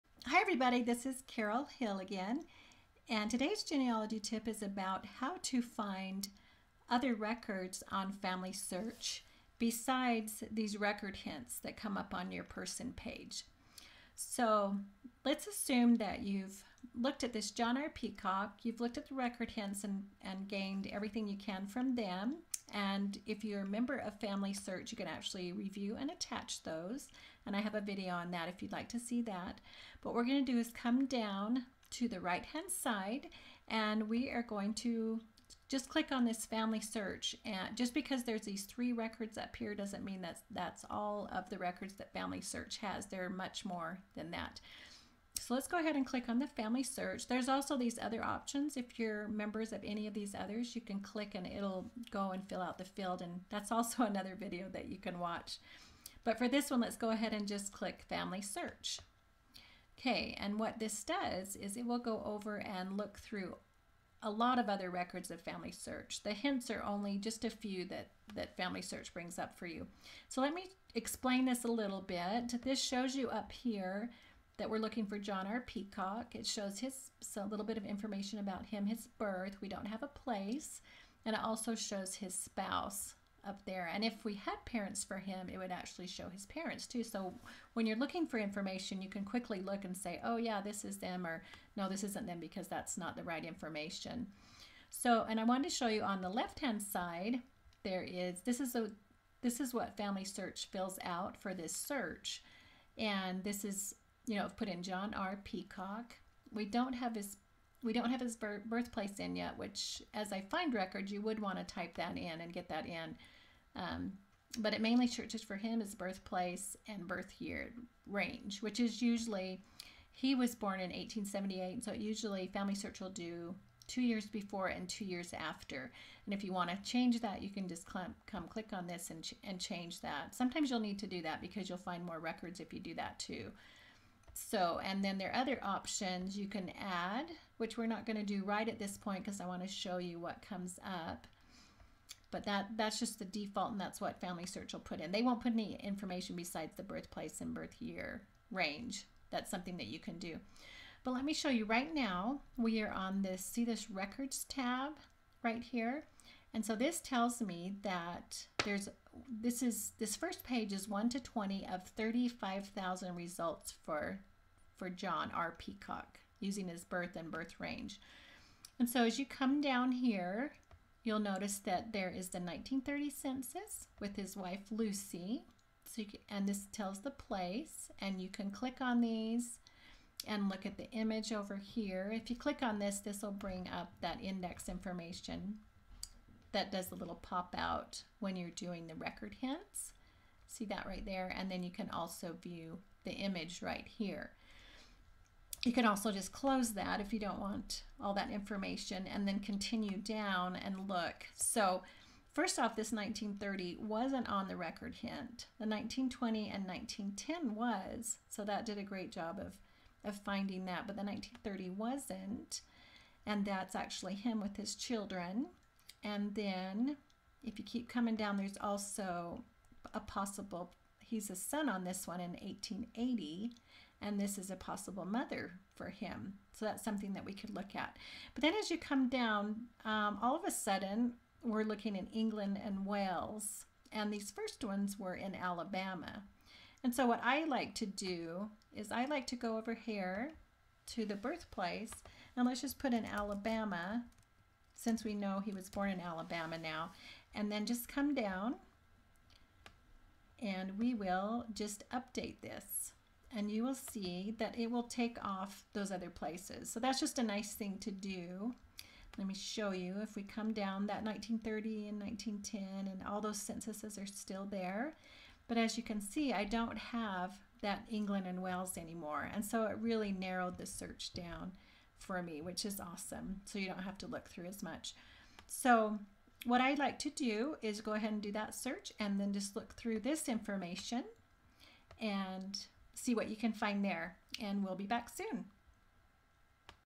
Step 3: How to Search for additional records from FamilySearch person page
So, you have looked at the Record Hints from your ancestors person page.  The next step to finding more records on FamliySearch is to click the FamliySearch icon on the person's page.  Watch this 4 minute video to learn how it works.